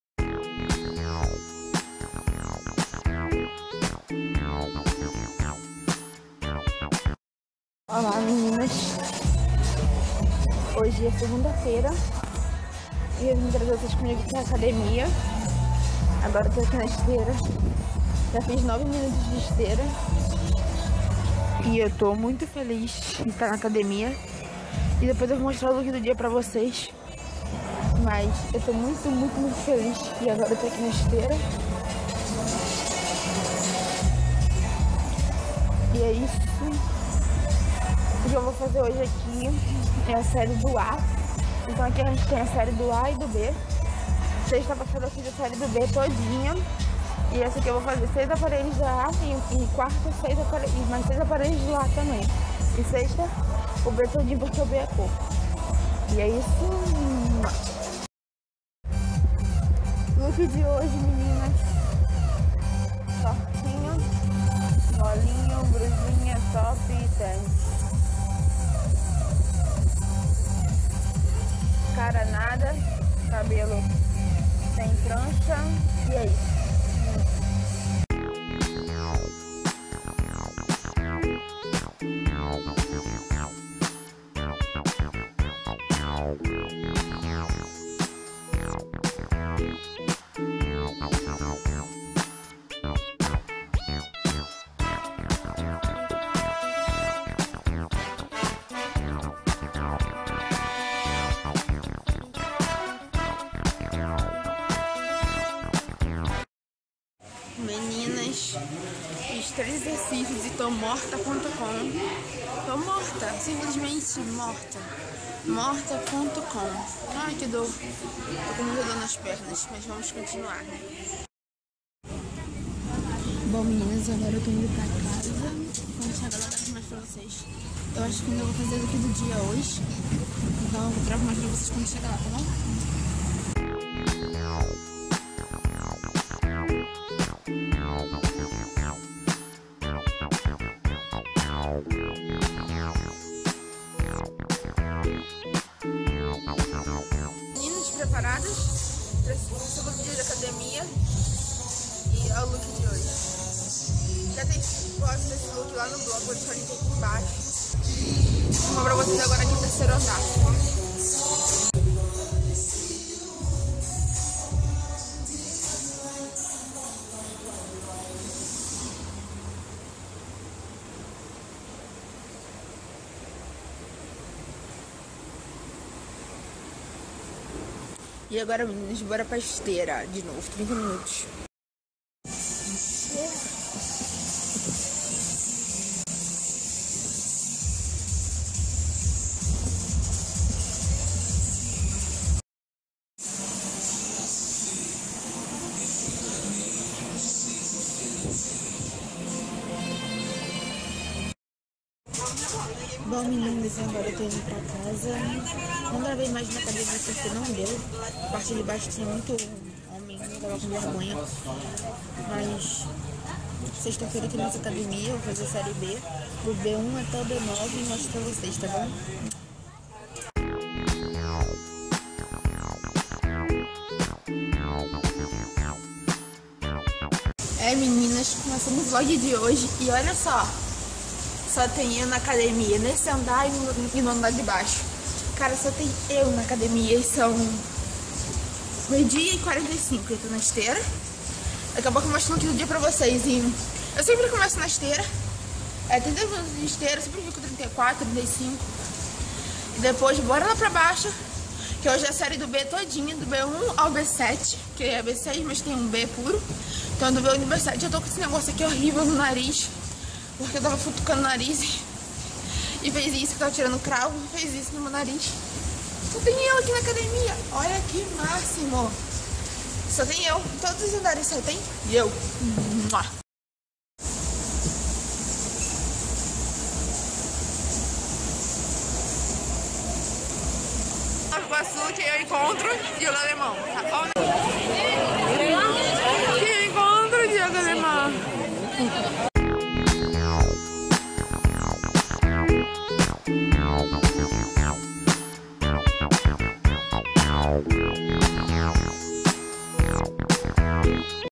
Vlog: Seg,qua,sex comigo na academia + Diego alemão em NI.
LEIA ABAIXO ;))) . Assista na maior qualidade 720 p HD .  ;)
Ignore os erros e os cortes ;(

 Mais informações .